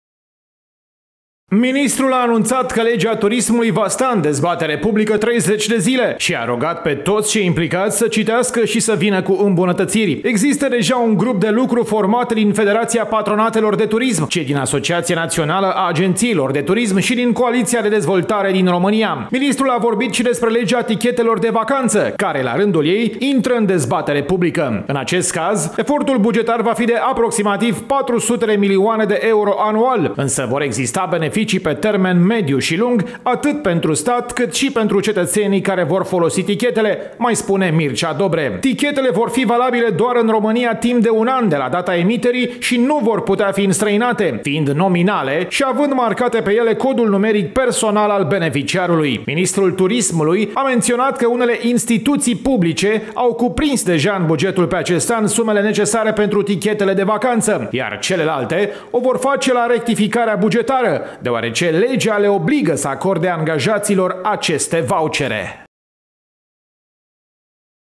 LEGEA TURISMULUI INTRĂ ÎN DEZBATERE PUBLICĂ
O lege bună în turism - asta este dorința ministrului de resort Mircea Titus-Dobre. Oficialul a anunțat că proiectul va intra in dezbatere publică săptămâna aceasta si că vrea ca mulți actori implicați să spună fiecare ce are de spus si să trimita materiale.